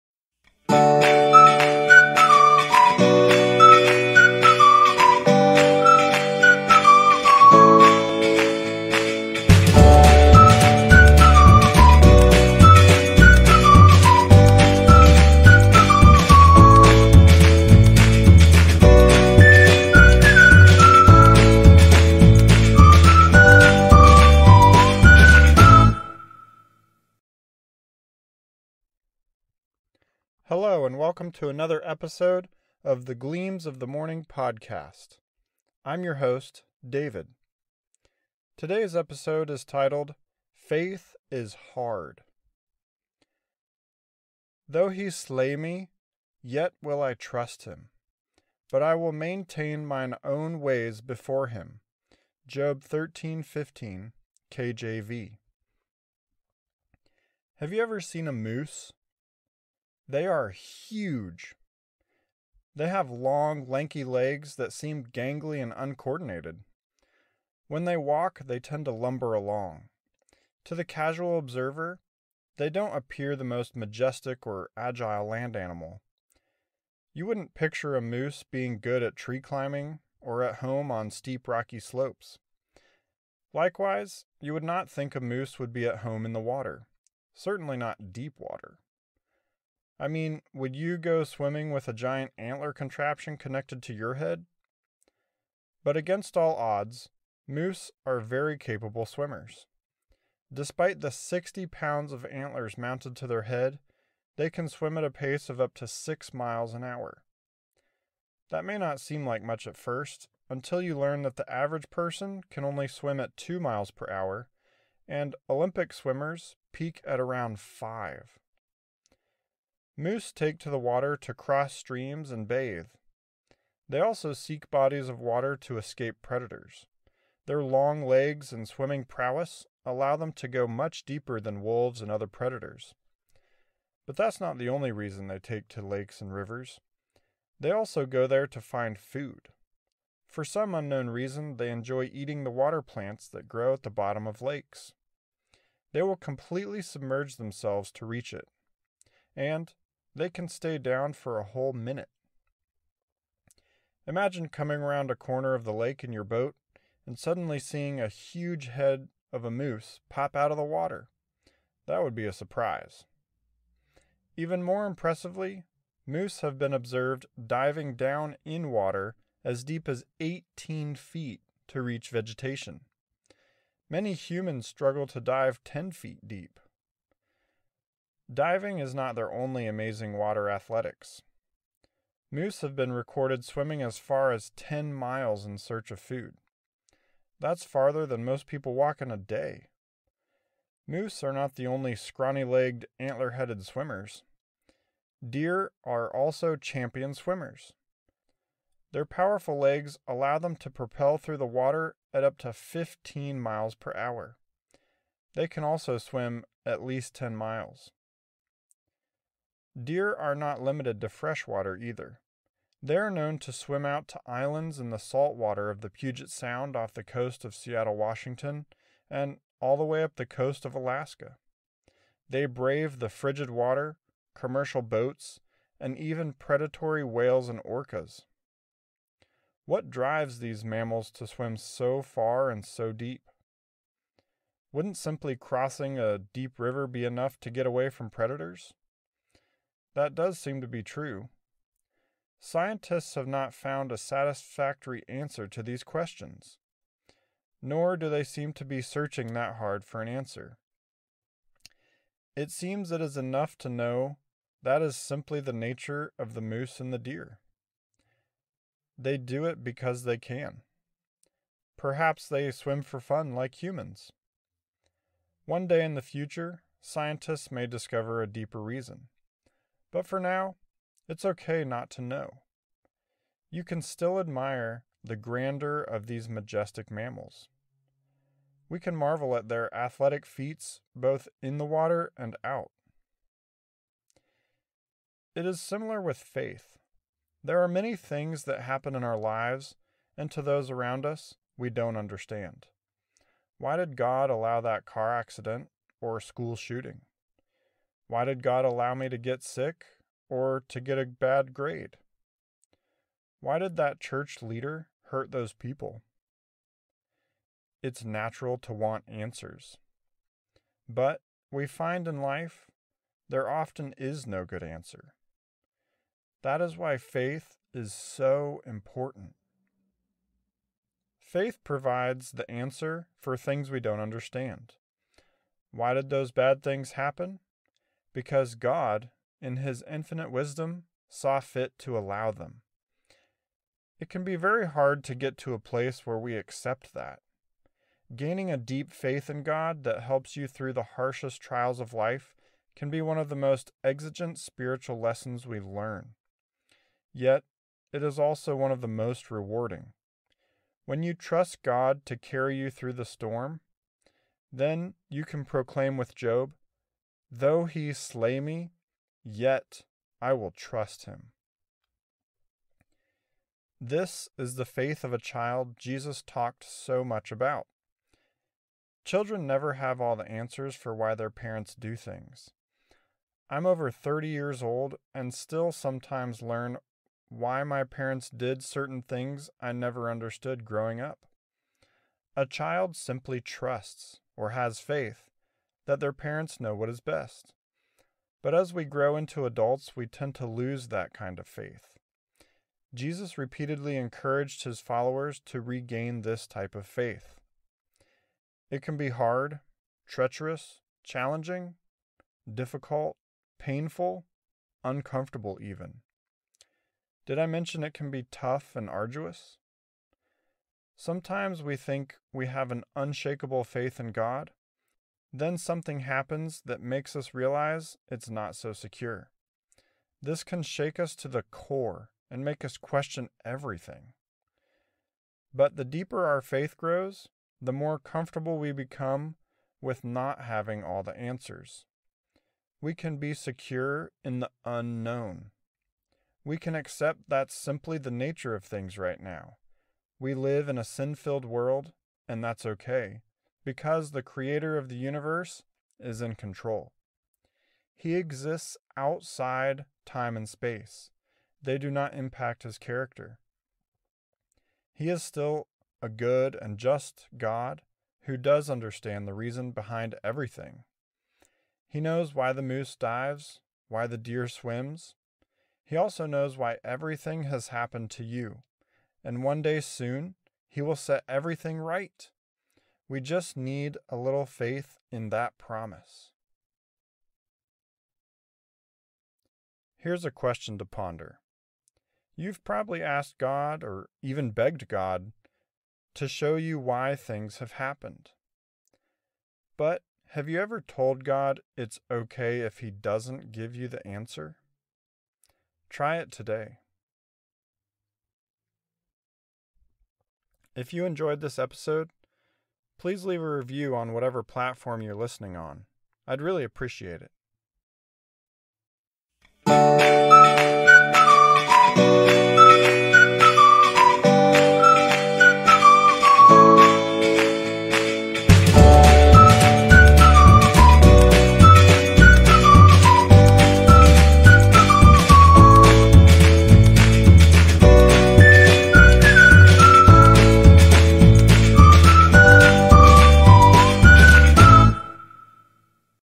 Faith is Hard - Episode 12
Have you ever struggled with your faith? It can be very hard to keep your faith in God when difficult things assail us. Yet we can grow to a deeper faith that will help us hold on through anything. Listen to this episode to hear more about how to grow your faith.